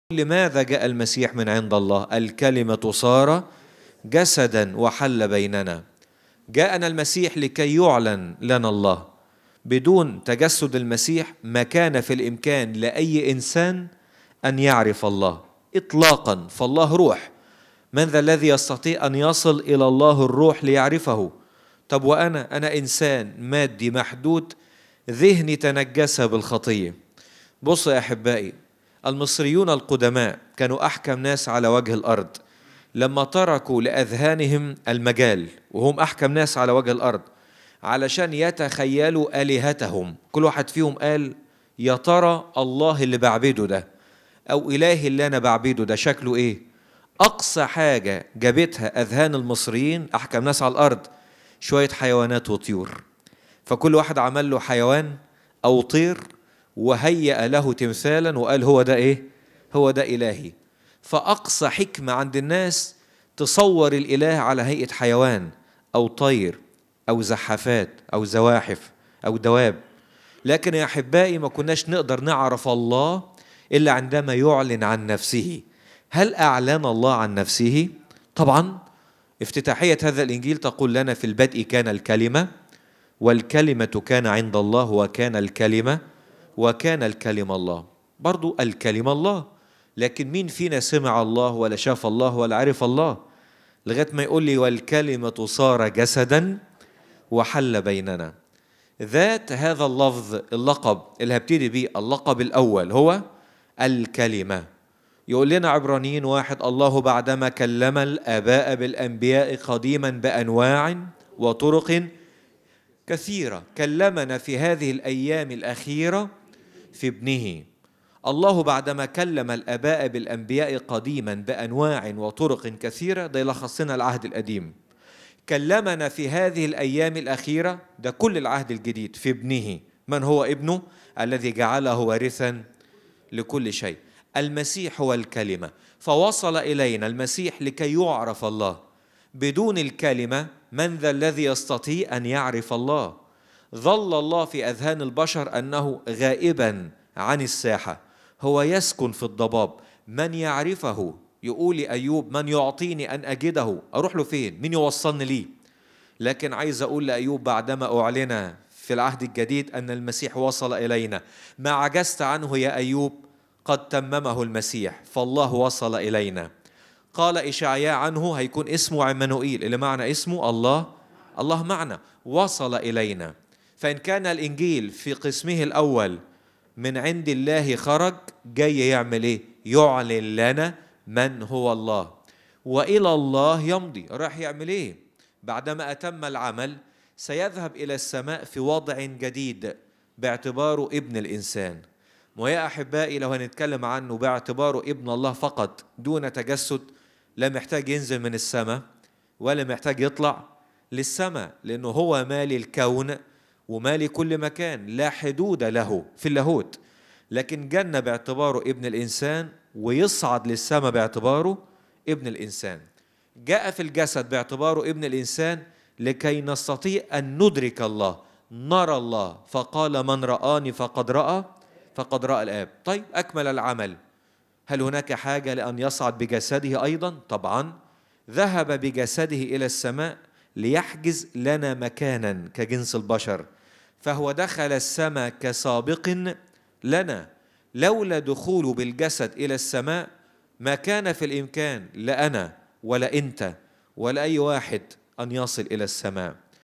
جائنا المسيح لكي يُعلَن لنا الله - الأخ عياد ظريف - اجتماع الحرية
اجتماع الحرية | ألقاب المسيح السبعة في يوحنا ١ - الأخ عياد ظريف | بث مباشرعلى المحطة الفضائية سات-7 يوم الاثنين 18 نوفمبر 2019 | انتاج سات-7 | اخراج جون عدلي Ayyad Zarif - Brethren Church Triumph Square - Freedom Meeting - SAT-7